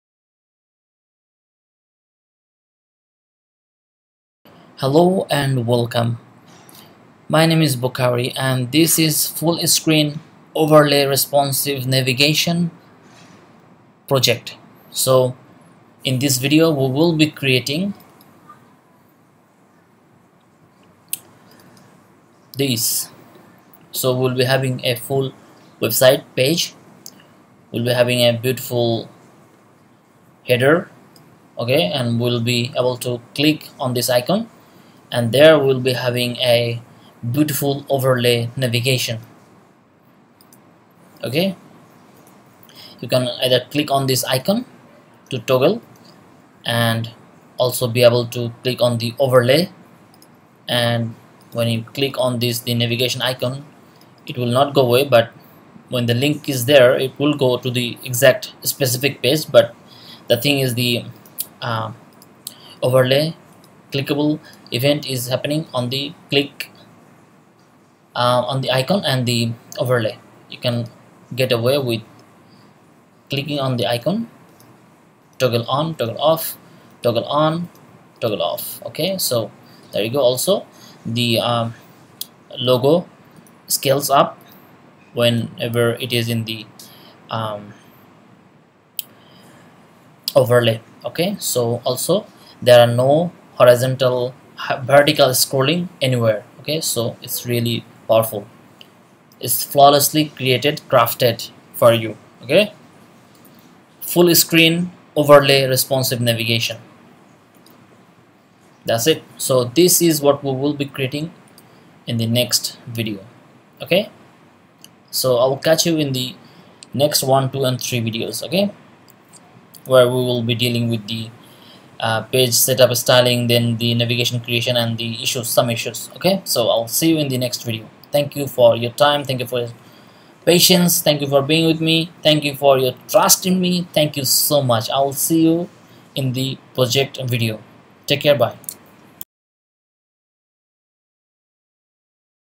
Fullscreen Overlay Responsive Navigation Intro
In this Fullscreen Overlay Responsive Navigation Project, I'll be showing you the technique of how to create or build a beautiful hand crafted Fullscreen Overlay Responsive Navigation using HTML5, CSS3 and jQuery.

In this Fullscreen Overlay Responsive Navigation Project, you'll learn and master the technique of creating the overlay effect and also be able to handle critical scrolling issue while in the overlay state.

Project Files will be available in my Github Repository.

Join LiveGood Today and Secure Future Financial Freedom.